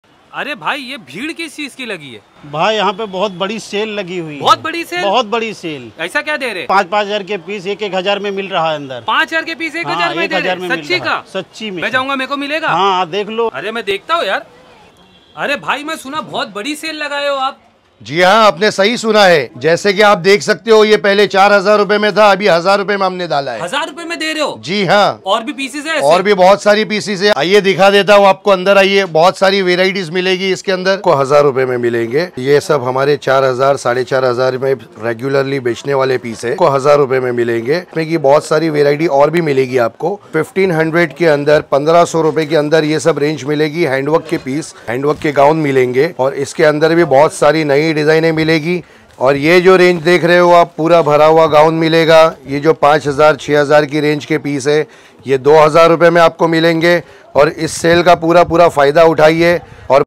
Mohammad Ali raod Ramdan special sale upto 70% off on partywear dresses,garara,gown,pakistanidress
Mohammad Ali Road nakhuda mohalla market biggest sale on latest partywear and fancy dresses